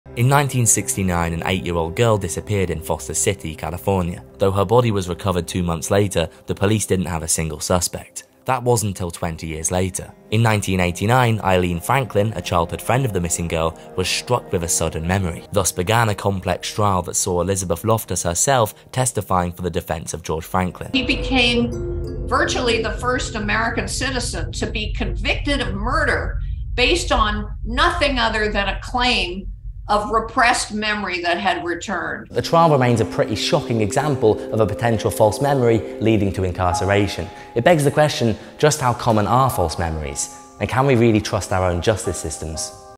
Can you really trust your own memory? Watch the full episode on our channel now!
Research has shown that 40% of people have a first memory that is fictional - we regularly contort our own pasts, but what if someone else could?